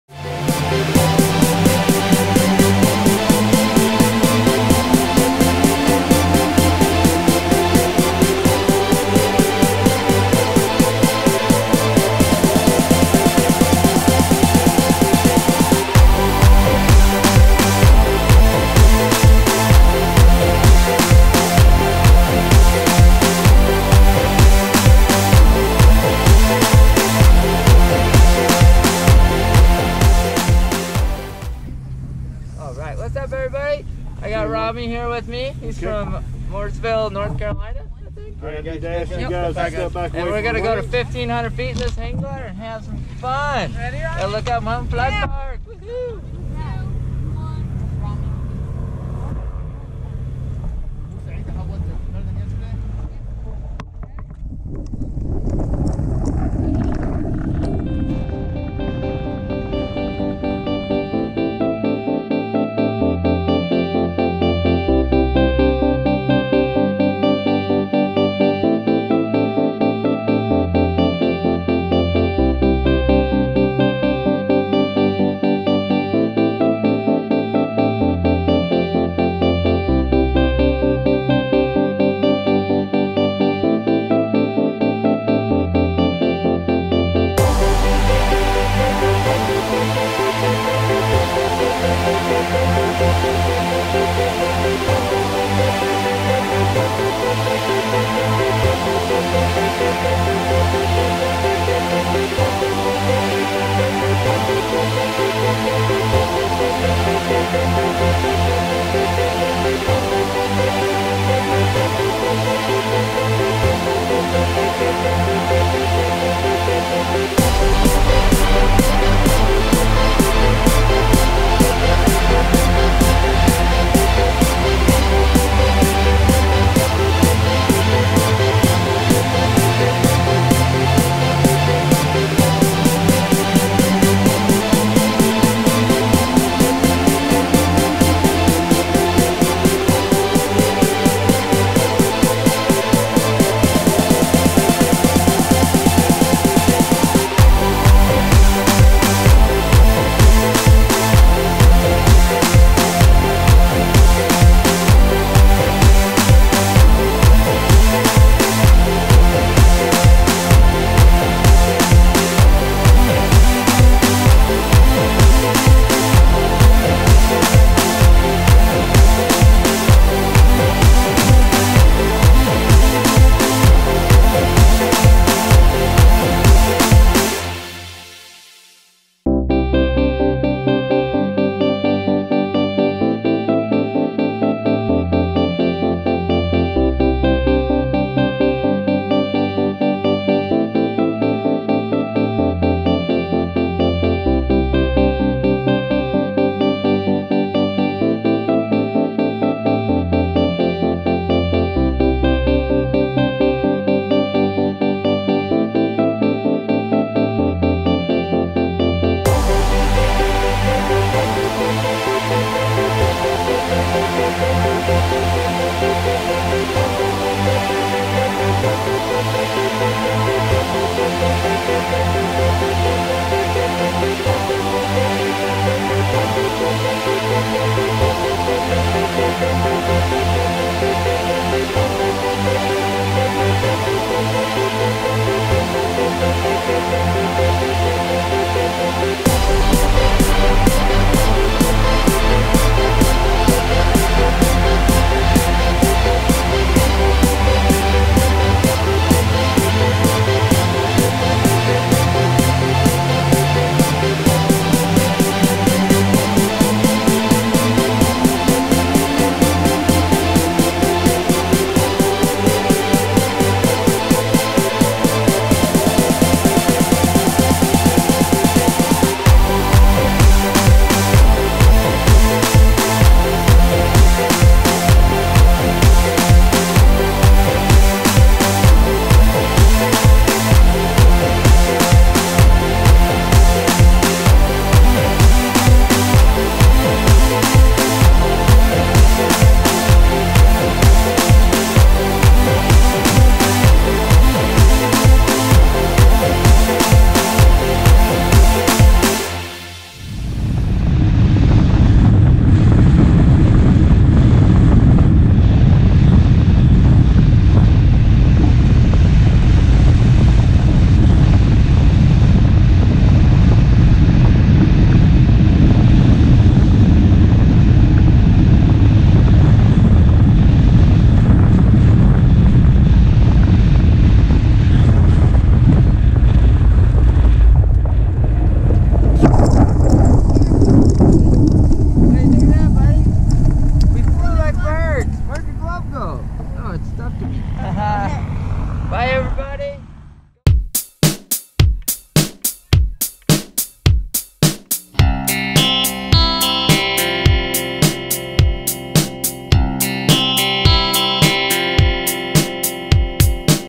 Rami Abdelbaky Tandem Hang Gliding at Lookout Mountain Flight Park
This video is about Rami Abdelbaky Tandem Hang Gliding at Lookout Mountain Flight Park